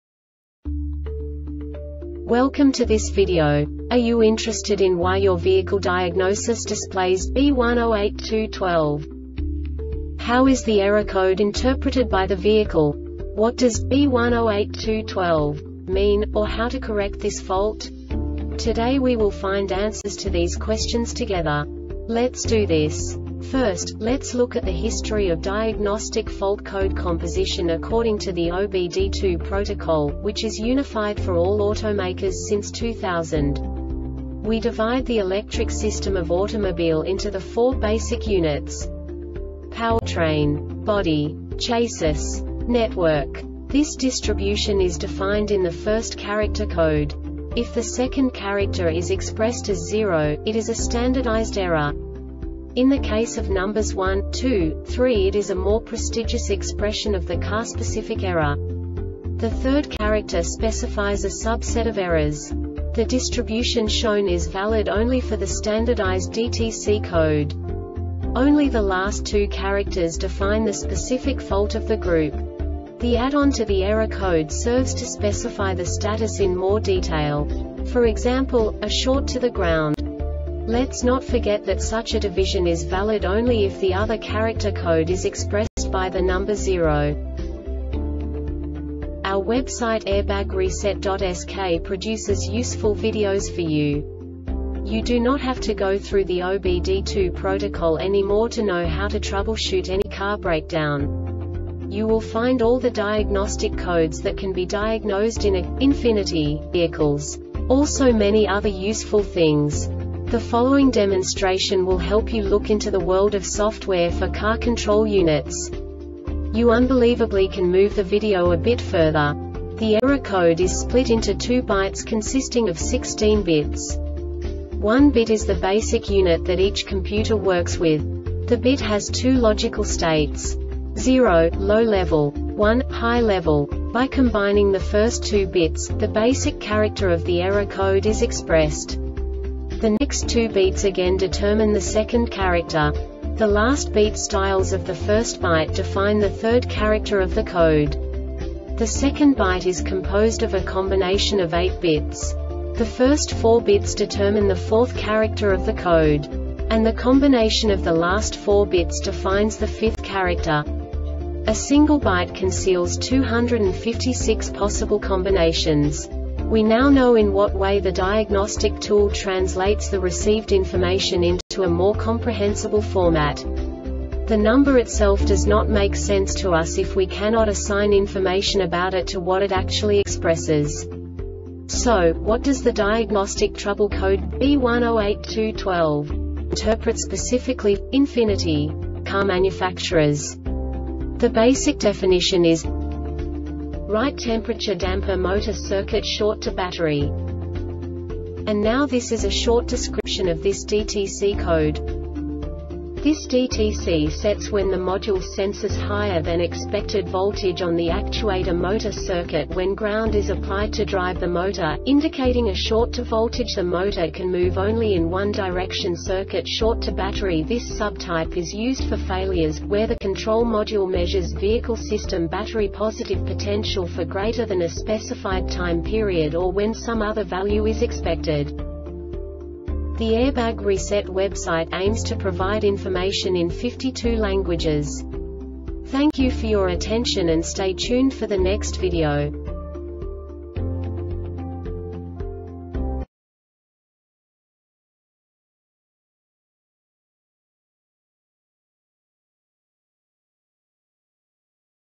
DTC Ford B1082-12 Short Explanation
Contents:
0:21 Basic DTC analysis according to OBD2 protocol standard.
1:48 Insight into programming
2:58 A diagnostically understandable description of the Diagnostic Trouble Code
3:05 Basic error definition

Make
Ford
Code
B108212
Definition
Right Temperature Damper Motor Circuit Short To Battery
Cause
This DTC sets when the module senses higher than expected voltage on the actuator motor circuit when ground is applied to drive the motor, indicating a short to voltage The motor can move only in one direction
Failure Type